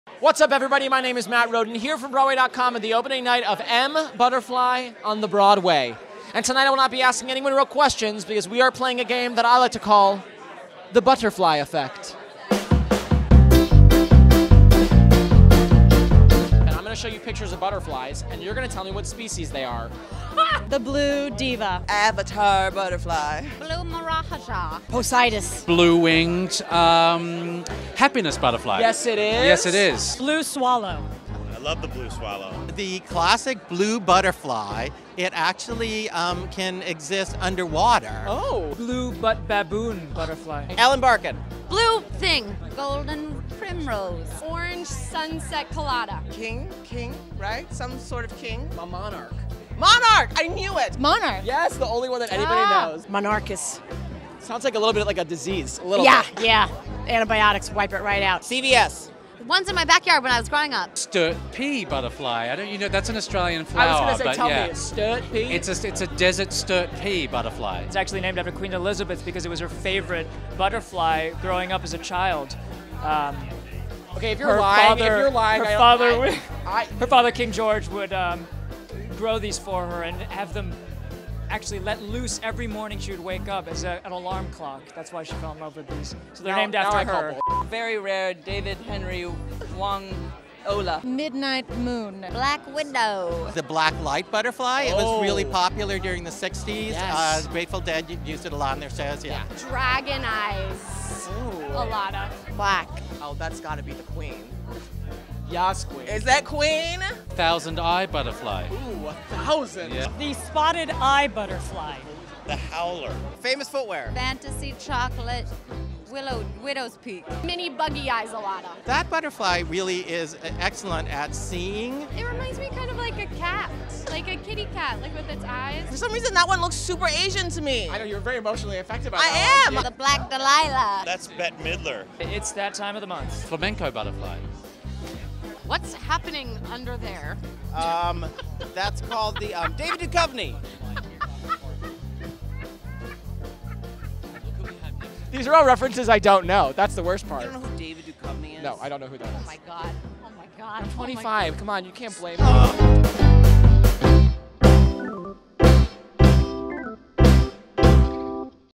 RED CARPET CHALLENGE: M. BUTTERFLY with Edie Falco, Andrew Rannells, and more!
Get Tickets to M. Butterfly

Matt Rodin challenges the cast and guests of M. BUTTERFLY to identify butterfly species. As always, hilarity ensues.